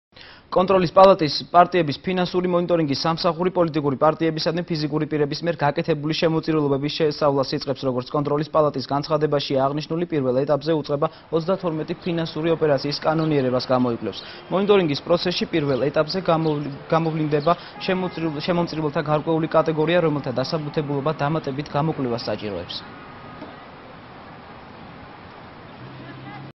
ახალი 6 | პარტიების მომიტორინგი | 24.06
კონტროლის პალატის პარტიების ფინანსური მონიტორინგის სამსახური პოლიტიკურ პარტიებისადმი ფიზიკური პირების მიერ გაკეთებული შემოწირულობების შესწავლას იწყებს. როგორც კონტროლის პალატის განცხადებაშია აღნიშნული, პირველ ეტაპზე უწყება 32 ფინანსური ოპერაციის კანონიერებას გამოიკვლევს. მონიტორინგის პროცესში I ეტაპზე გამოვლინდა შემოწირულებათა გარკვეული კატეგორია, რომელთა დასაბუთებულობა დამატებით გამოკვლევას საჭიროებს.